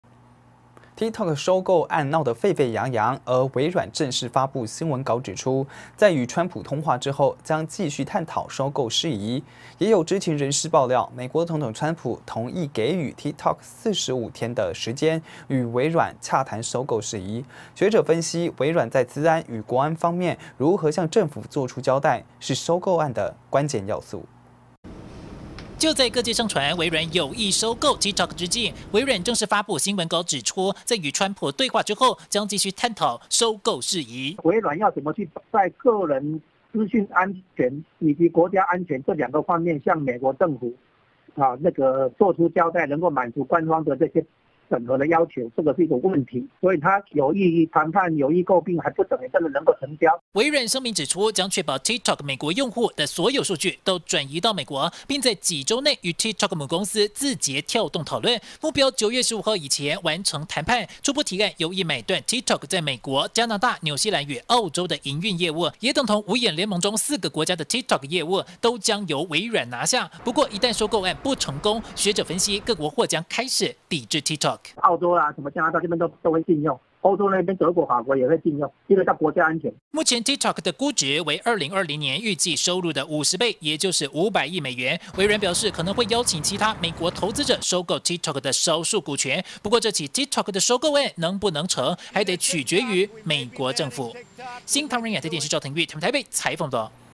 微軟：續論TikTok收購案 有意拿下美加紐澳業務
TikTok收購案鬧得沸沸揚揚，而微軟正式發布新聞稿指出，在與川普通話之後，將繼續探討收購事宜，也有知情人士爆料，美國總統川普同意給予TikTok四十五天的時間與微軟洽談收購事宜。學者分析，微軟在資安與國安方面如何向政府做出交代，是收購案的關鍵要素。